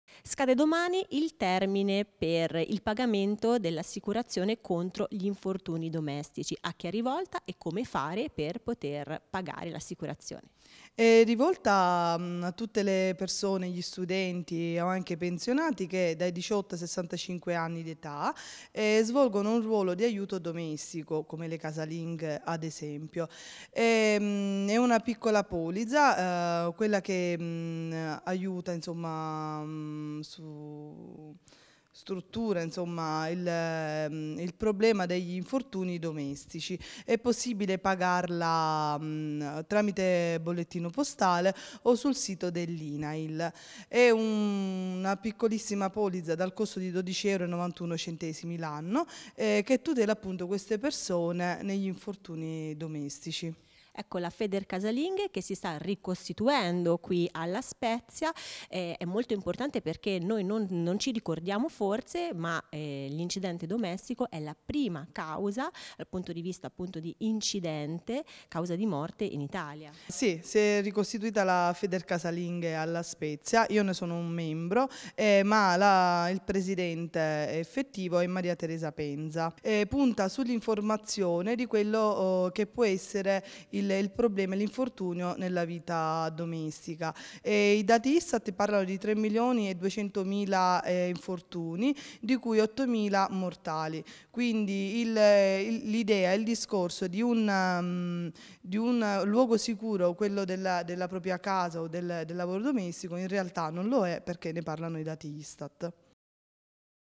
Assicurazione obbligatoria casalinghe, il 31 la scadenza 30-01-2017
Scade domani il termine per il pagamento dell’ assicurazione obbligatoria contro gli infortuni domestici, un rischio spesso sottovalutato, sono infatti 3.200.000 gli incidenti che si verificano ogni anno in Italia nelle nostre abitazioni, di cui 8.000 con esito mortale: la prima causa di morte in Italia per incidenti, davanti a incidenti sul lavoro e stradali.